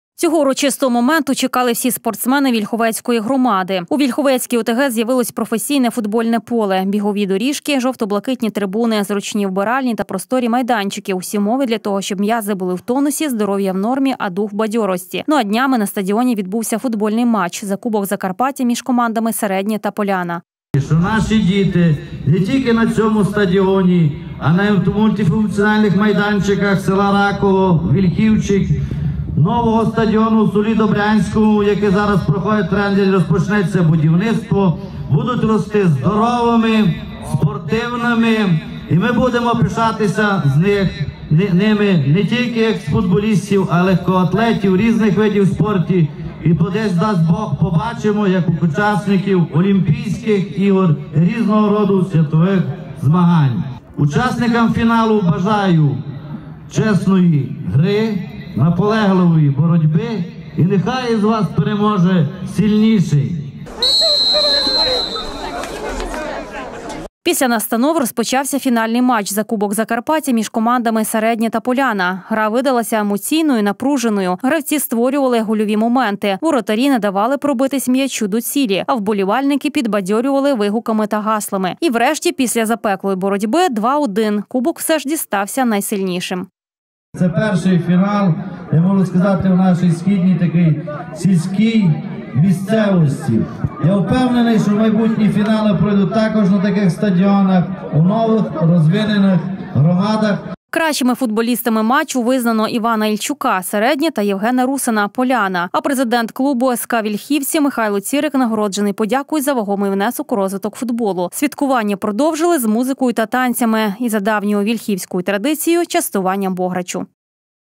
Футбольний матч. Нове професійне поле
У Вільховецькій ОТГ з`явилось  професійне футбольне поле, бігові доріжки, жовто-блакитні трибуни, зручні вбиральні та просторі майданчики - всі умови для того, щоб м’язи були в тонусі, здоров’я в нормі а дух в бадьорості. Ну а днями на стадіоні відбувся футбольний матч   за Кубок Закарпаття між командами “Середнє” та “Поляна”.  Цього урочистого моменту чекали всі спортсмени Вільховецької громади, 14 жовтня відкрилася нова сторінка місцевого спортивного життя.  Після настанов в такій урочистій атмосфері розпочався фінальний матч за Кубок Закарпаття між командами “Середнє” та “Поляна”.  Гра видалася емоційною, напруженою, Гравці створювали гольові моменти, воротарі не давали пробитись м’ячу до цілі, а вболівальники підбадьорювали вигуками і гаслами. І врешті після запеклої боротьби 2:1 - кубок все ж дістався найсильнішим. . Кращими футболістами матчу визнано: Івана Ільчука (Середнє) та Євгена Русина (Поляна), дитячо-юнацька команда “Вільхівці” отримала м’ячі для розвитку футболу, а президент клубу СК “Вільхівці” - Михайло Цірик нагороджений подякою за вагомий внесок у розвиток футболу. Святкування продовжились музиками, танцями та за давньою вільхівською традицією - частуванням бограчу.